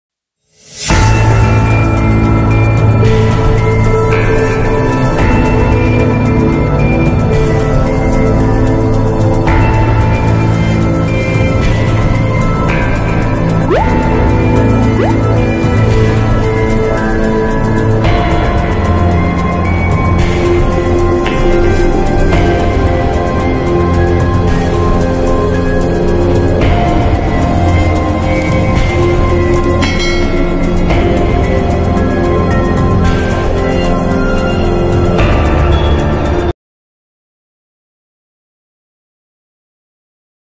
It's real scary zombie hand on 😱🤯 google earth 🌍
I found zombie hand sculpture on google maps and google earth 🌍 @Thegoogleearth.09